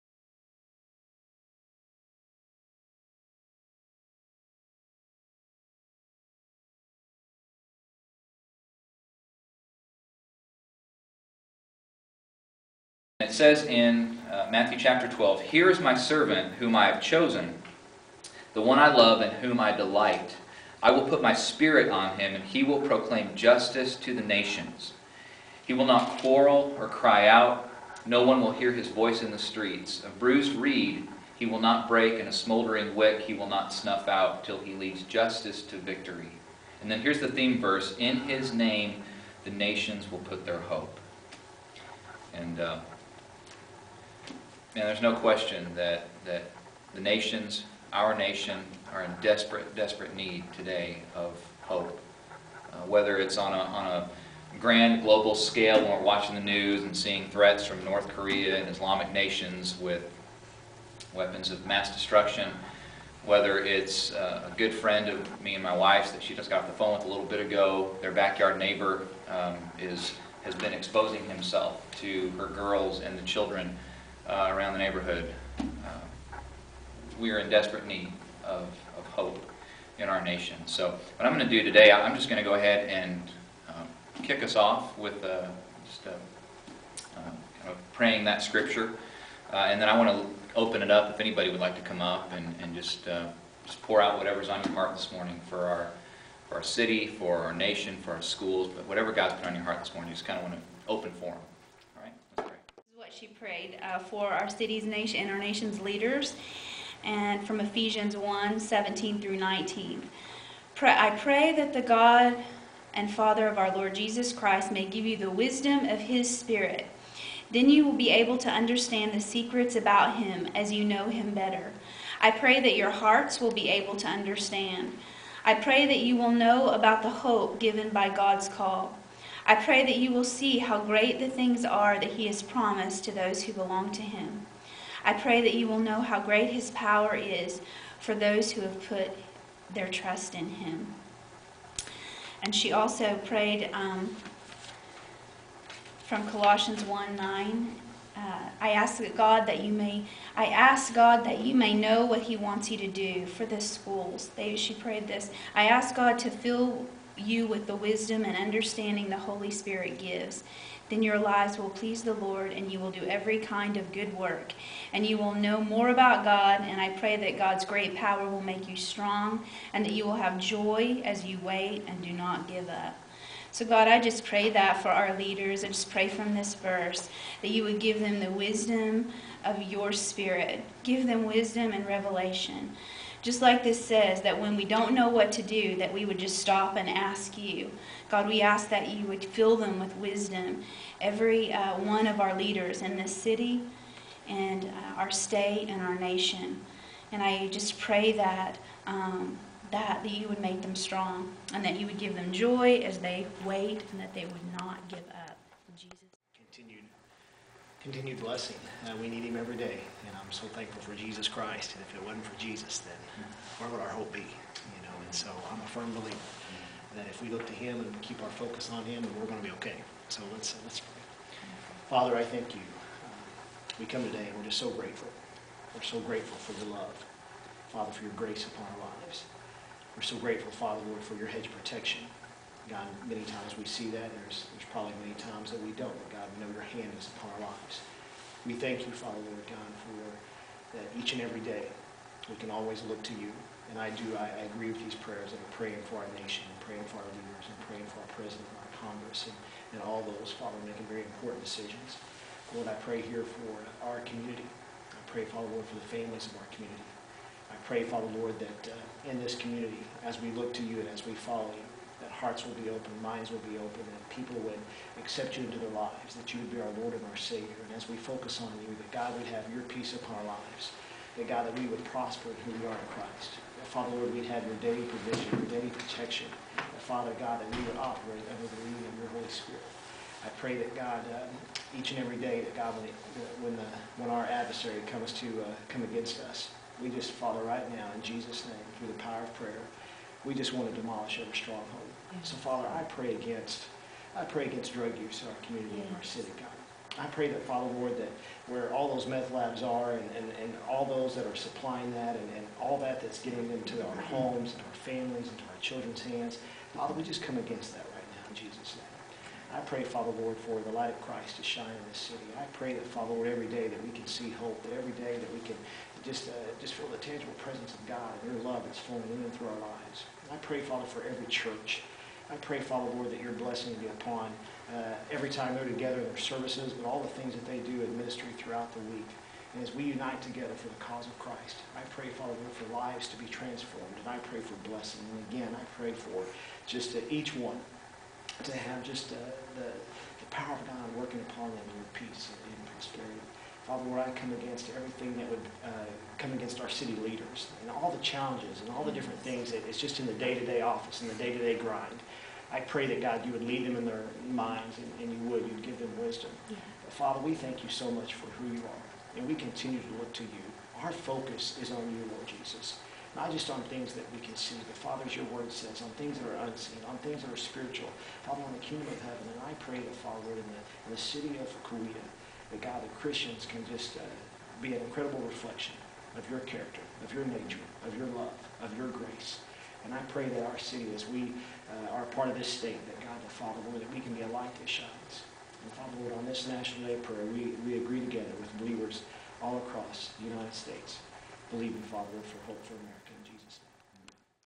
National Day of Prayer in Coweta
Inclement weather moved Coweta's National Day of Prayer gathering indoors Thursday where several local pastors shared their thoughts and prayers for our community, state and nation.